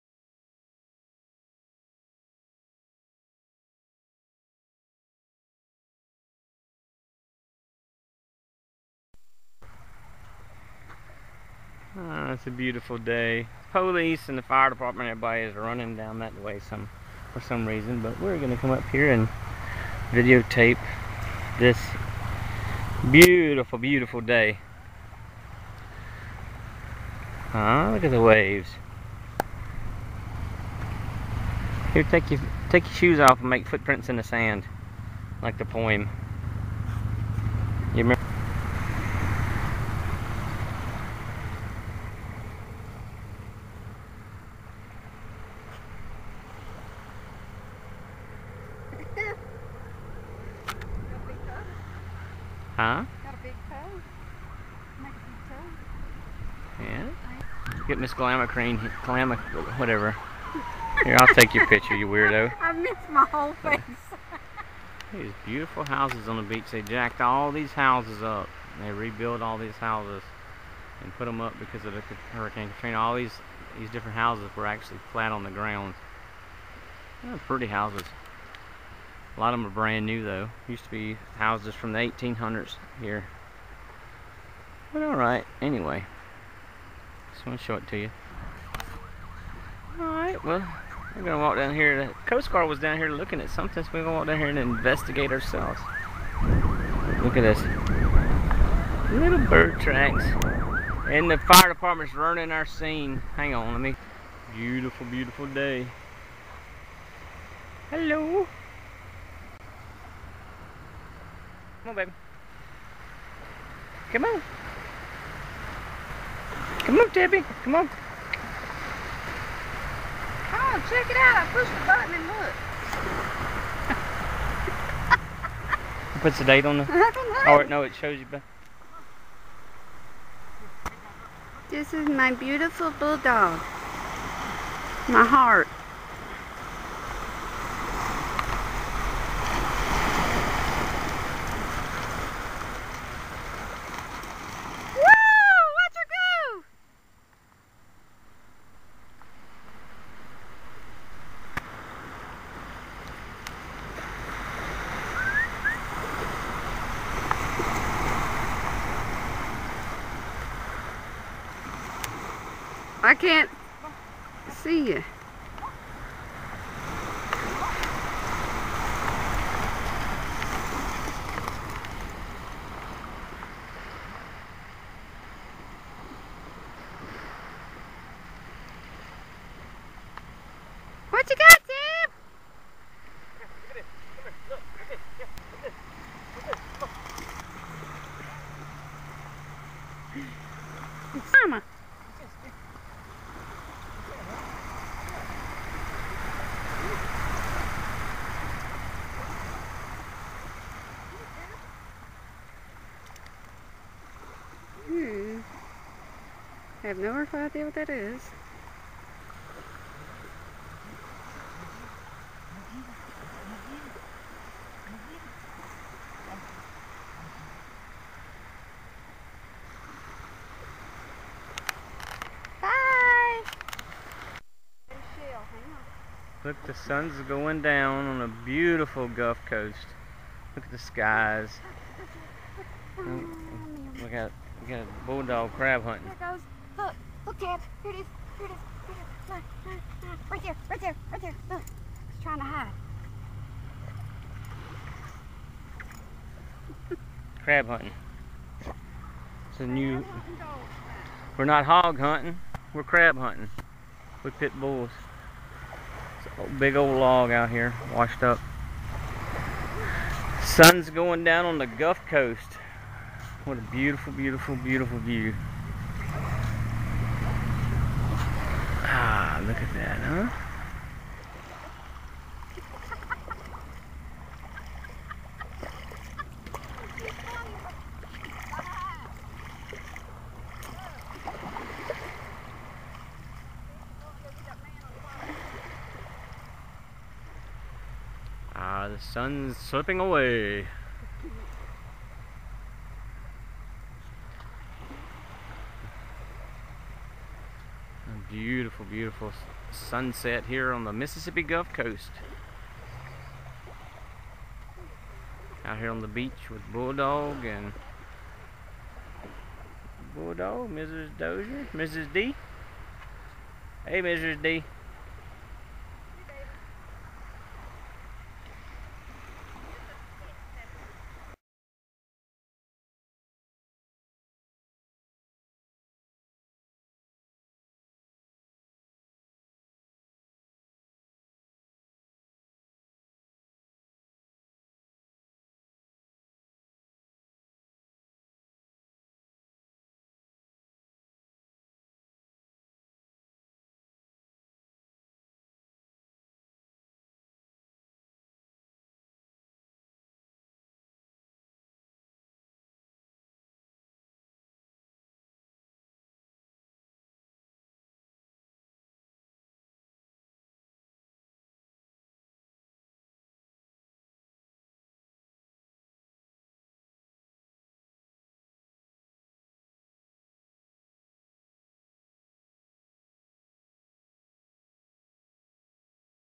PITBULL FUN
WALK ON THE BEACH WITH THE GIRLS ON THE BEACH